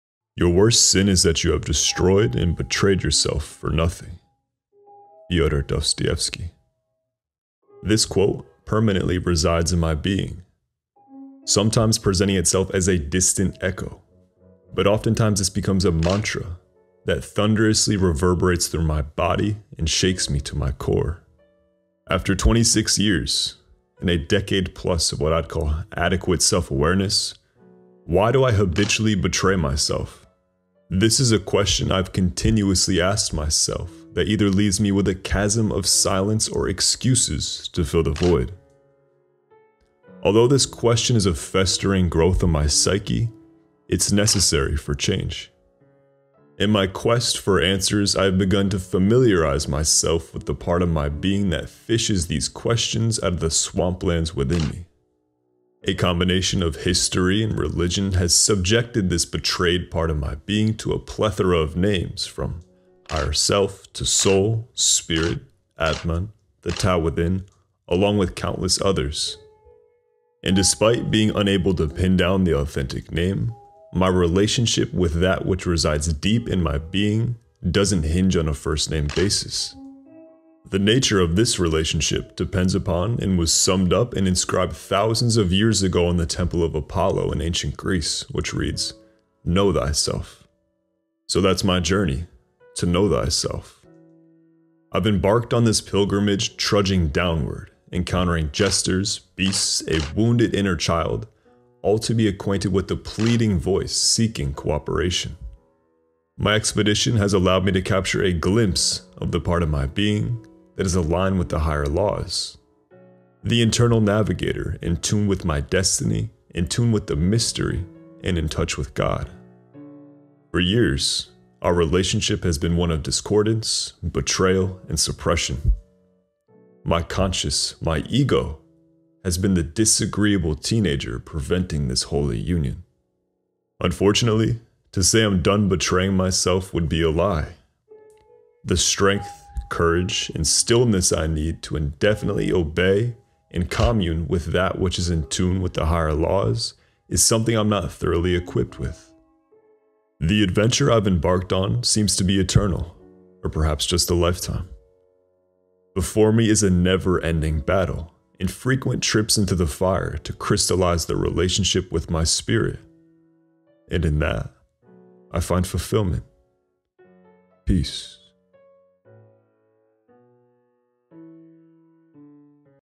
How To Stop Betraying Yourself
As Thoreau put it we often trudge about our days “living quiet lives of desperation”. When night time falls we sit with ourselves engulfed in a dissatisfaction that not even a slumber can keep at bay. Where does the constriction of the innards come from? Why does our one precious life seem so burdensome? Oftentimes the discontent stems from the betrayal of self. Navigating your depths and plummeting beneath the surface can reveal to you your innermost being that's in touch with the mystery of life and God. Frequently we find ourselves out of touch and removed from our spirit and essence but put your ear a little closer to the ground because the call towards the higher laws never dies.